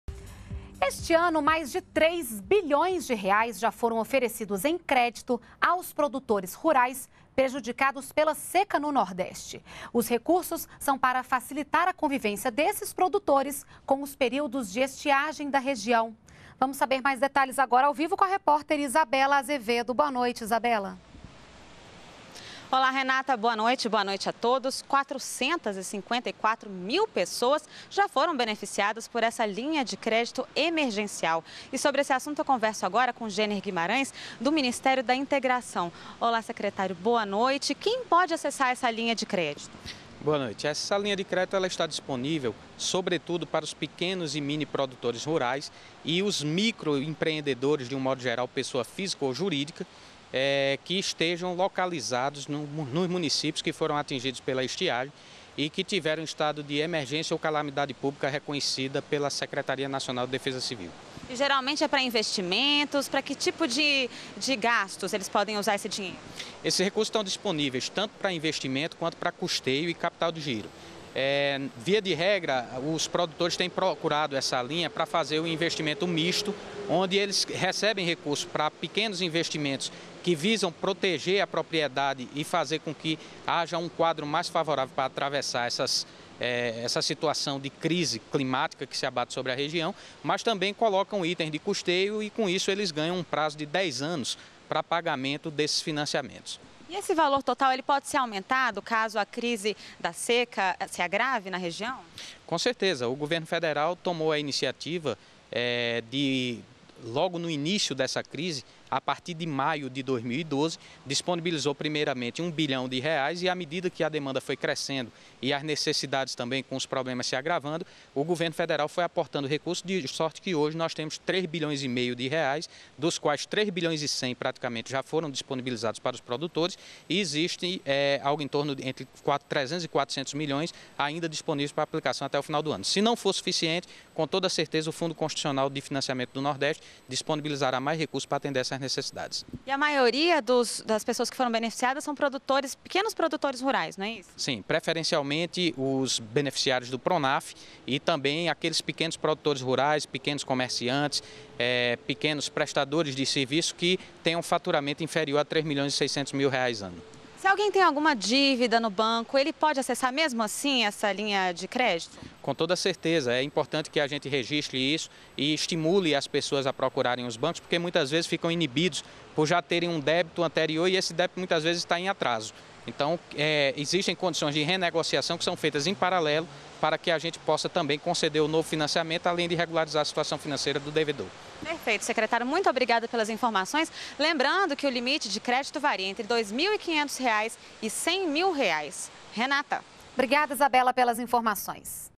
Produtores rurais prejudicados pela seca tiveram R$ 3 bilhões em crédito em 2013
NBR NOTÍCIAS - 30.10.13: Mais de R$ 3 bilhões já foram oferecidos em crédito aos produtores rurais prejudicados pela seca no Nordeste. Os recursos são para facilitar a convivência desses produtores com os períodos de estiagem da região. A linha de crédito emergencial, destinada principalmente a pequenos produtores e microempreendedores rurais, beneficiou 454 mil pessoas. O limite para o financiamento varia entre R$ 2,5 mil e R$ 100 mil. Mais detalhes na entrevista com o secretário de Fundos regionais e Incentivos Fiscais do Ministério da Integração, Jenner Guimarães.